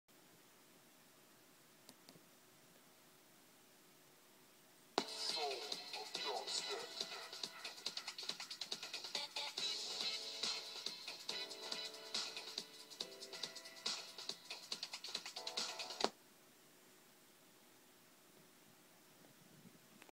Raspberry Pi as Dubstep Internet radio pt2
My project to build a Raspberry Pi based internet radio that plays only Dubstep now does not need a keyboard or monitor anymore thanks to the two small switches on the breadboard connected to the GPIO pins via the Adafruit board.  The buttons control a Python script.